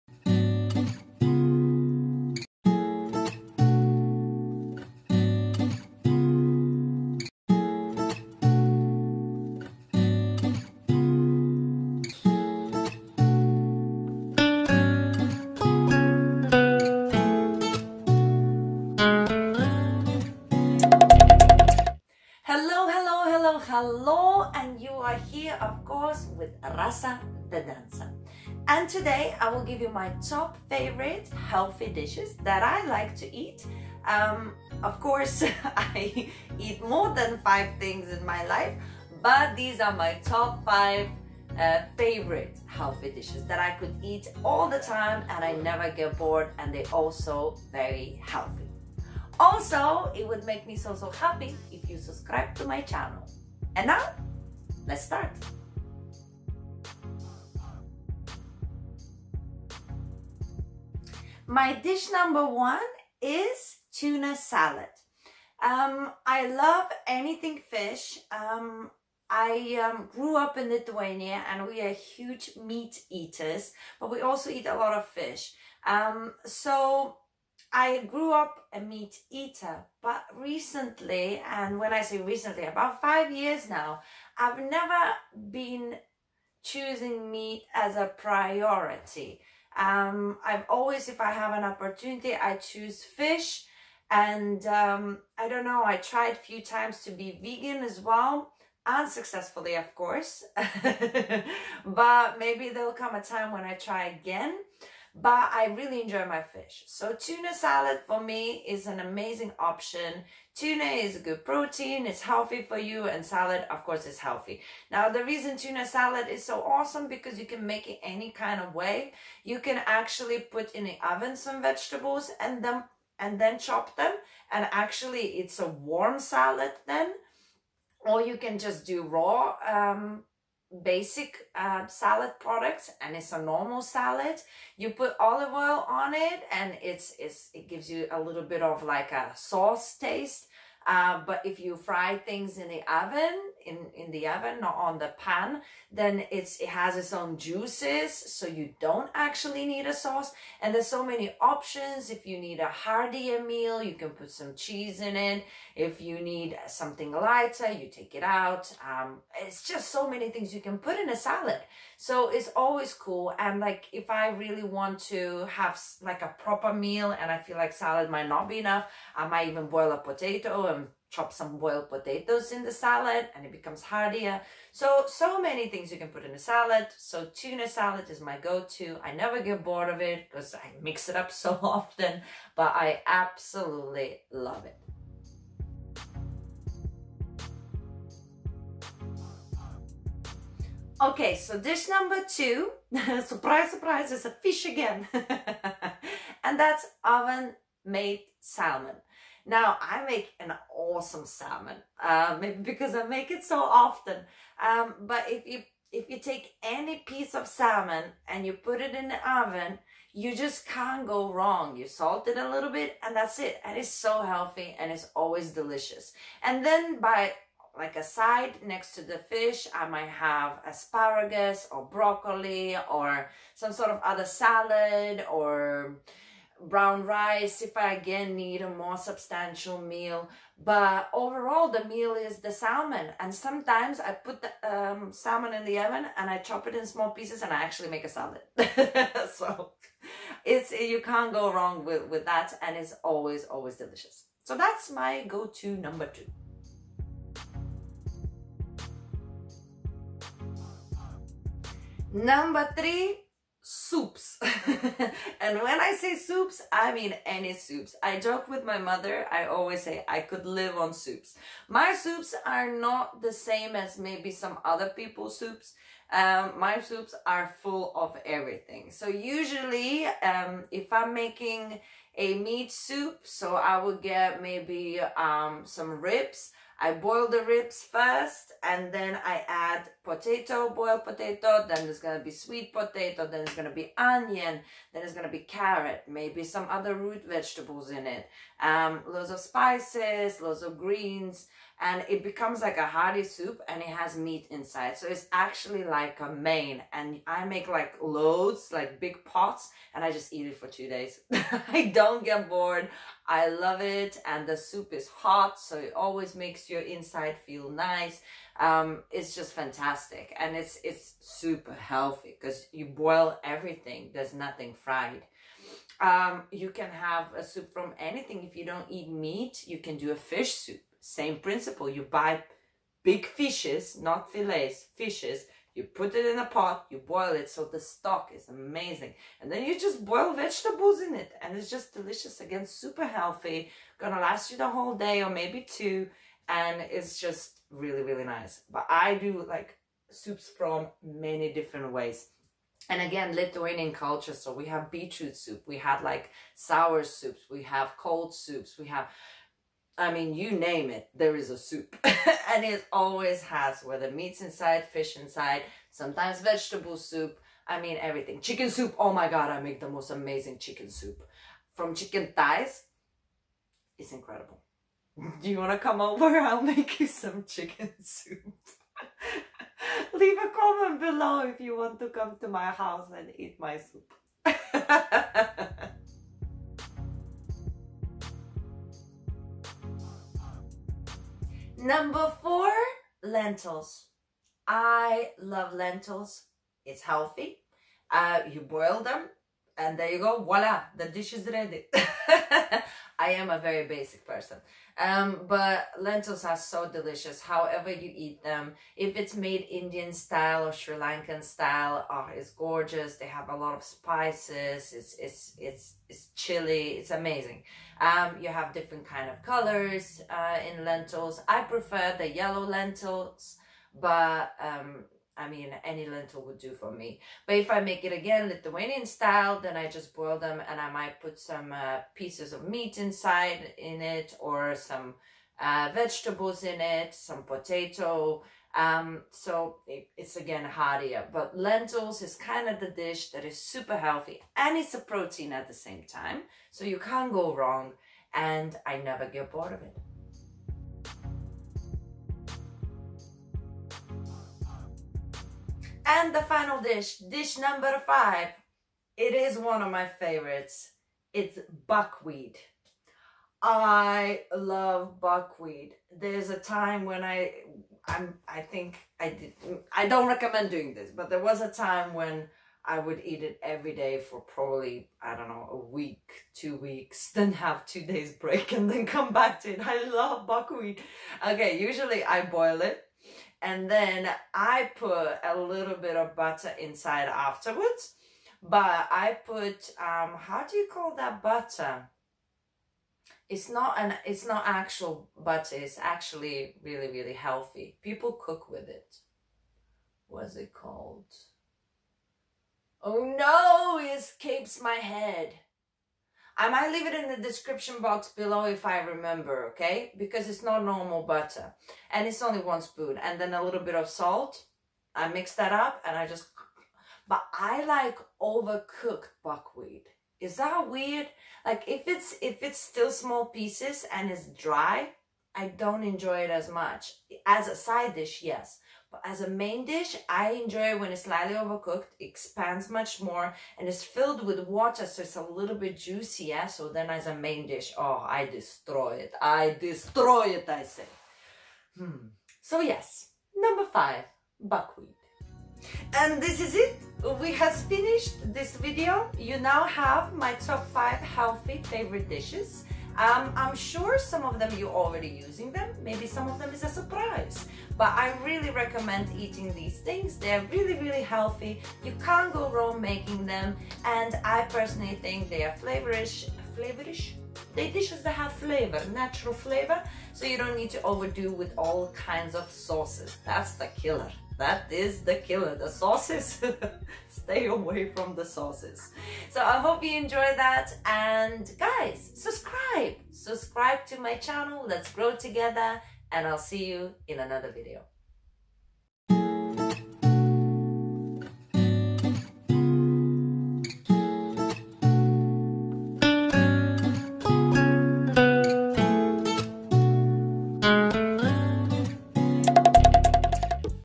Top 5 Favourite Dishes | What I Eat | Healthy Lifestyle - Rasa Pauzaite
Rasa Pauzaite - Top 5 Favourite Dishes | What I Eat | Healthy Lifestyle - Rasa Pauzaite

Today I'm talking food! This week, I thought it would be interesting to tell you my five favourite dishes to eat! I eat a wide variety of food in my diet, but these are the dishes I could eat time and time again without getting bored. I hope they inspire you.

It makes me feel special and encourages me to share more of my life with you!